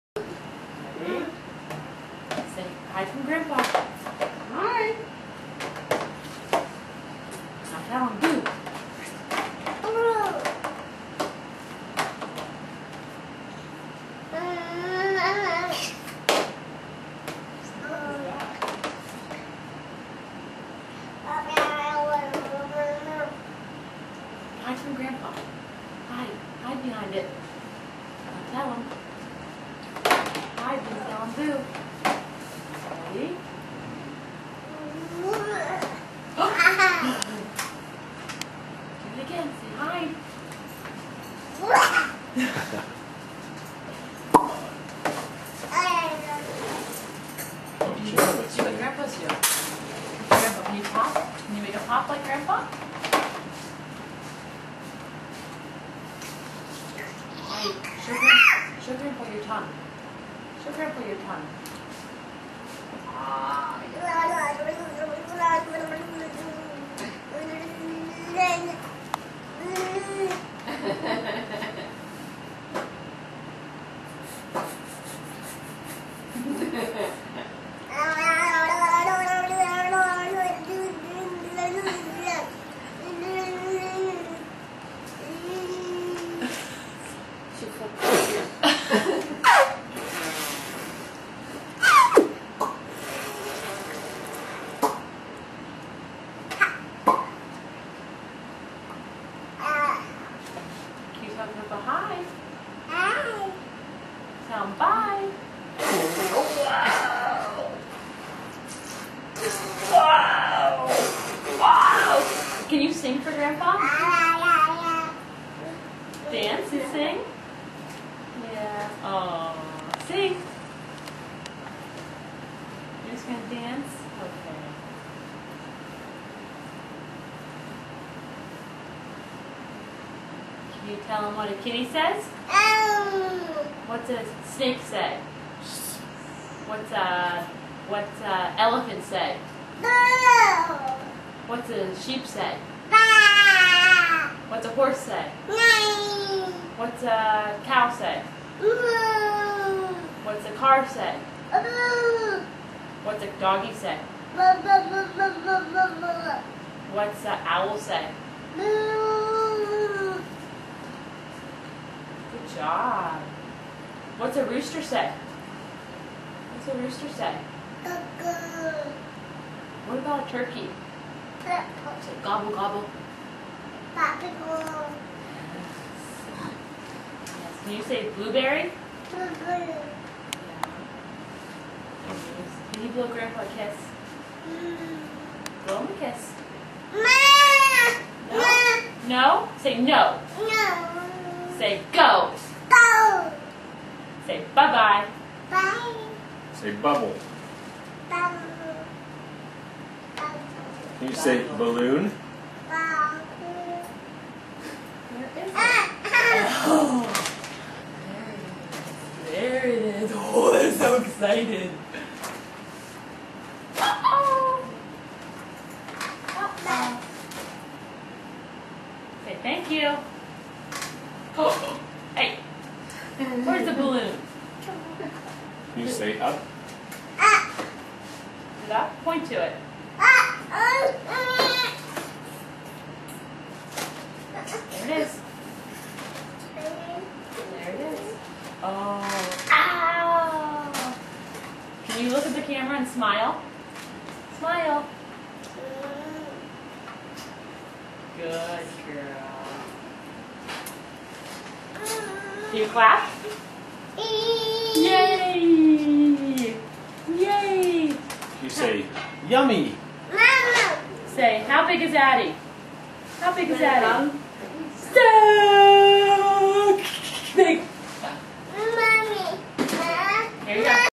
Adalyn after dinner
Adalyn talks to us from her high chair